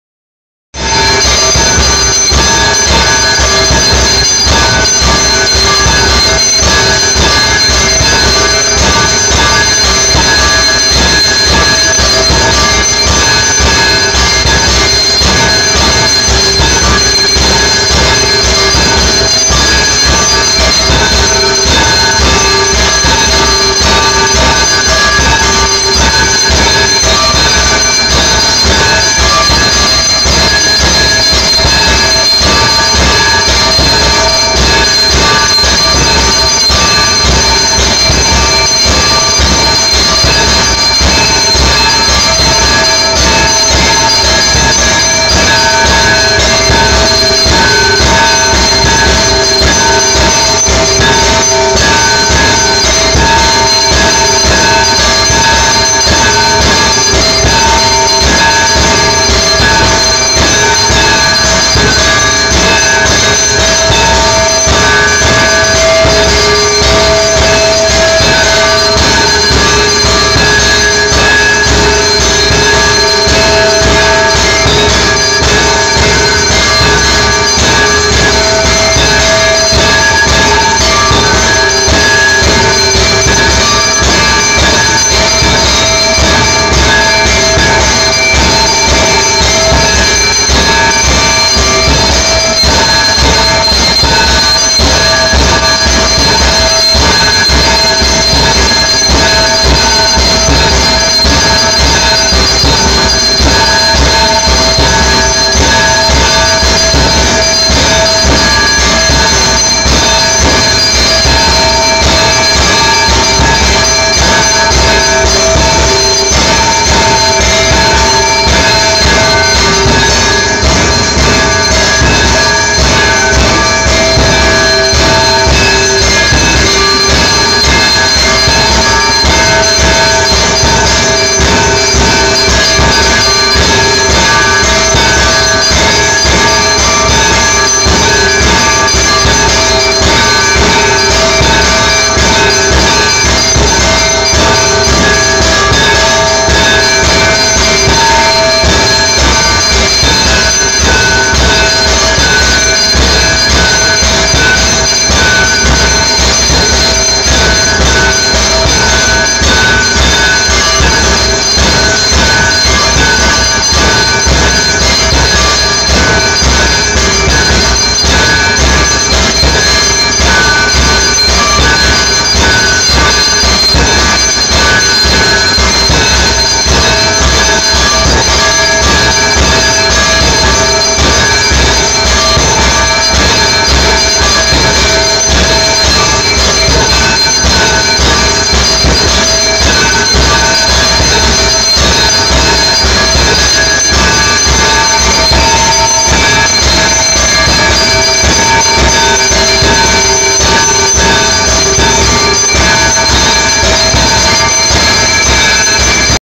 Bhurakhiya Hanuman mandir aarti video 5th july 2012
Bhurakhiya aarti video 5th july 2012 early morning aarti , Bhurakhiya Hanuman Mandir , vijay parekh and family